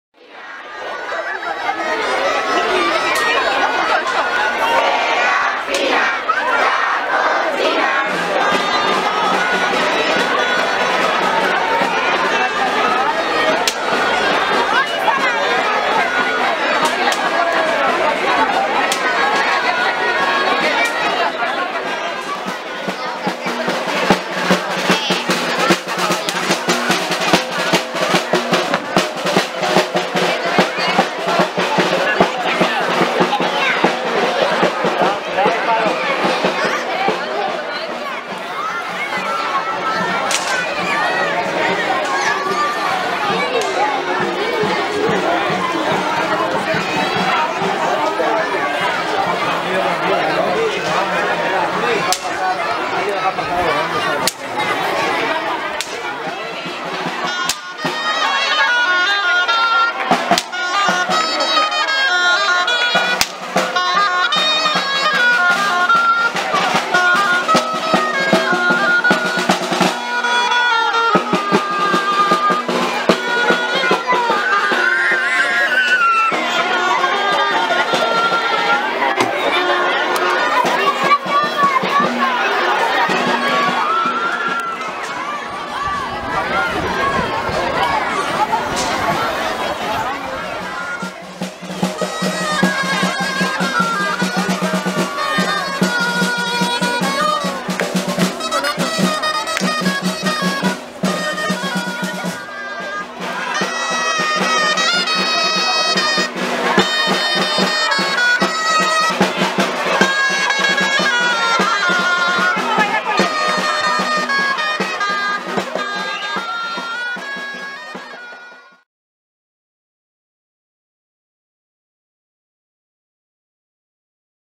De paseo con la charamita (07/12/2016)
Un paseo con la charamita por las calles de Torrevieja con motivo de las fiestas patronales.